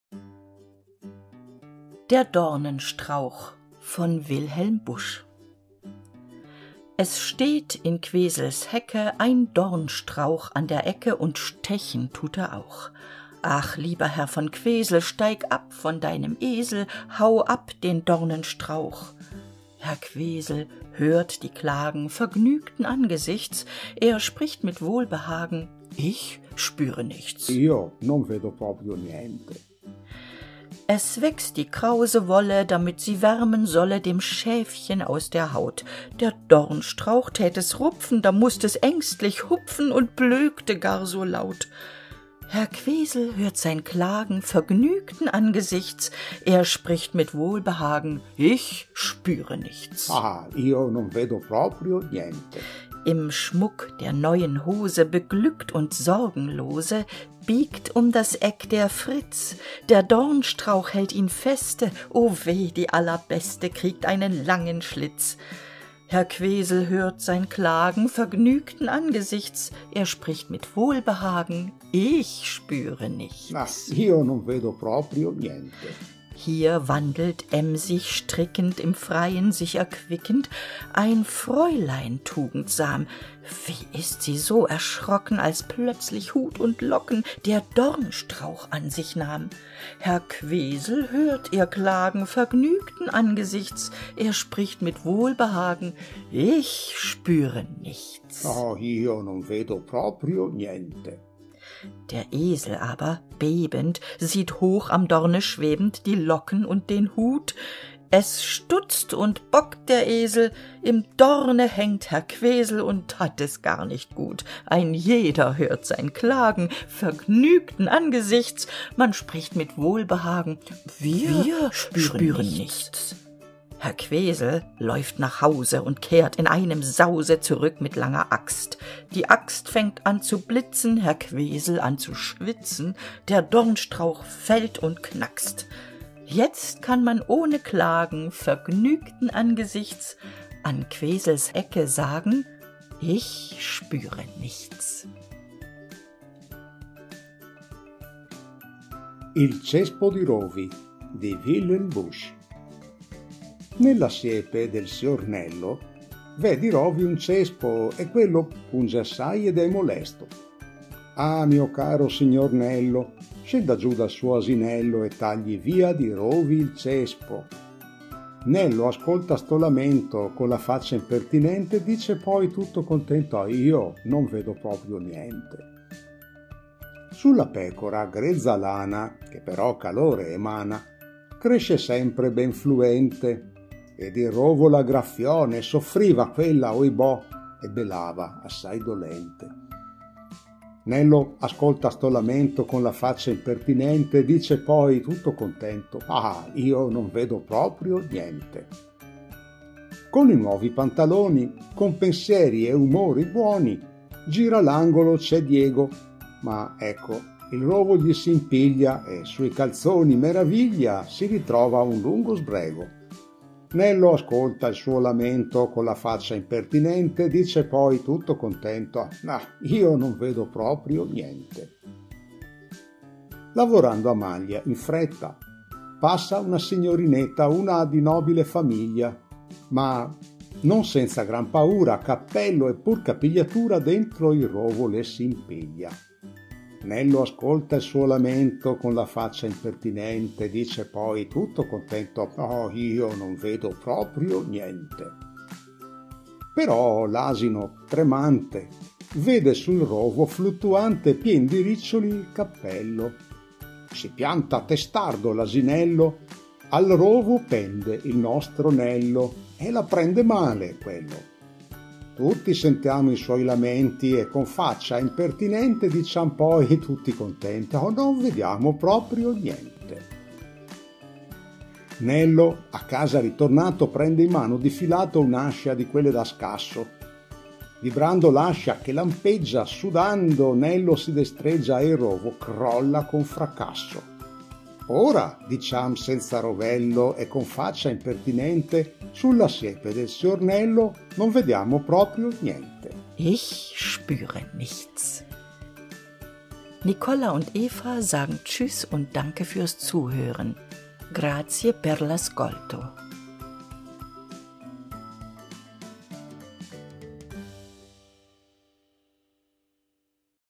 Der Dornenstrauch / Il cespo di rovi - di Wilhelm Busch, traduzione Nicola Curcio
00:00 deutsch
02:30 italiano

Der Dornenstrauch
   von Wilhelm Busch

Es steht in Quesels Hecke
Ein Dornstrauch an der Ecke,
Und stechen tut er auch.
Ach, lieber Herr von Quesel,
Steig ab von deinem Esel,
Hau ab den Dornenstrauch.
Herr Quesel hört die Klagen
Vergnügten Angesichts.
Er spricht mit Wohlbehagen:
Ich spüre nichts!

Es wächst die krause Wolle,
Damit sie wärmen solle
Dem Schäfchen aus der Haut.
Der Dornstrauch tät es rupfen,
Da mußt es ängstlich hupfen
Und blökte gar so laut.
Herr Quesel hört sein Klagen
Vergnügten Angesichts.
Ich spüre nichts!

Im Schmuck der neuen Hose,
Beglückt und sorgenlose
Biegt um das Eck der Fritz.
Der Dornstrauch hält ihn feste,
O weh, die Allerbeste
Kriegt einen langen Schlitz.
Vergnügten Angesichts.
Ich spüre nichts!

Hier wandelt, emsig strickend,
Im Freien sich erquickend,
Ein Fräulein tugendsam.
Wie ist sie so erschrocken,
Als plötzlich Hut und Locken
Der Dornstrauch an sich nahm.
Herr Quesel hört ihr Klagen
Vergnügten Angesichts.
Ich spüre nichts!

Der Esel aber bebend
Sieht hoch am Dorne schwebend
Die Locken und den Hut.
Es stutzt und bockt der Esel,
Im Dorne hängt Herr Quesel
Und hat es gar nicht gut.
Ein jeder hört sein Klagen
Vergnügten Angesichts.
Man spricht mit Wohlbehagen:
Wir spüren nichts.

Herr Quesel läuft nach Hause
Und kehrt in einem Sause
Zurück mit langer Axt.
Die Axt fängt an zu blitzen,
Herr Quesel an zu schwitzen,
Der Dornstrauch fällt und knackst.
Jetzt kann man ohne Klagen
Vergnügten Angesichts
An Quesels Ecke sagen:
Ich spüre nichts.

Il cespo di rovi
   di Wilhelm Busch

Nella siepe del sior Nello
v’è di rovi un cespo, e quello
punge assai ed è molesto.
Ah, mio caro signor Nello
scenda giù dal suo asinello,
e tagli via di rovi il cespo.
Nello ascolta ’sto lamento
con la faccia impertinente.
Dice poi tutto contento:
io non vedo proprio niente!

Sulla pecora grezza lana,
che però calore emana,
cresce sempre ben fluente.
Ed il rovo la graffiò,
ne soffriva quella, ohibò,
e belava, assai dolente.

Con i nuovi pantaloni,
con pensieri e umore buoni,
gira l’angolo; c’è Diego.
Ma ecco, il rovo gli s’impiglia,
e sui calzoni, meraviglia,
si ritrova un lungo sbrego.
Nello ascolta il suo lamento

Lavorando a maglia, in fretta,
passa una signorinetta,
una di nobile famiglia.
Ma, non senza gran paura,
cappello e pur capigliatura
dentro il rovo le s’impiglia.

Però l’asino, tremante,
vede sul rovo, fluttuante,
pien di riccioli, il cappello.
si pianta, testardo, l’asinello,
al rovo pende il nostro Nello,
e la prende male, quello.
Tutti sentiamo i suoi lamenti,
e, con faccia impertinente,
diciam poi, tutti contenti:
non vediamo proprio niente!

Nello, a casa ritornato,
prende in mano, difilato,
un’ascia, di quelle da scasso.
Vibrando l’ascia, che lampeggia,
sudando, Nello si destreggia,
e il rovo crolla con fracasso.
Ora diciam, senza rovello,
sulla siepe del sior Nello

traduzione: Nicola Curcio

Bild: KI-generiert mit dem Image Creator von Microsoft Bing

Vortrag und Bearbeitung. Eva K. Kühn und Nicola Curcio